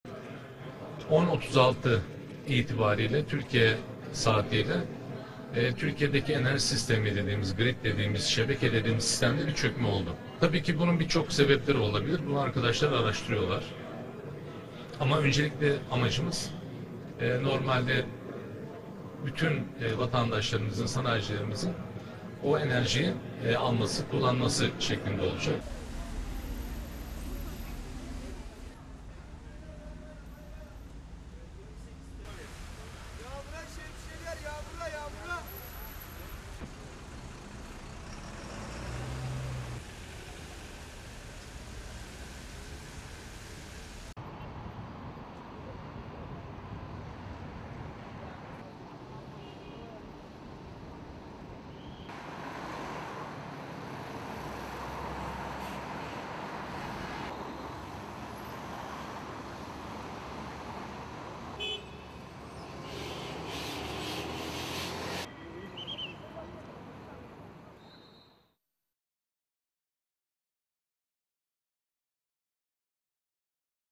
Türkiye'de elektrik kesintileri nedeniyle hayat durdu - BBC TÜRKÇE
Türkiye son yıllardaki en büyük genel elektrik kesintilerinden birini yaşıyor.

Enerji Bakanı Taner Yıldız Sabah 10:36'dan itibaren etkili olan kesintinin nedeninin "sistemdeki bir çökme" olduğunu söyledi.

Bazı illere elektrik verilmeye başlandı.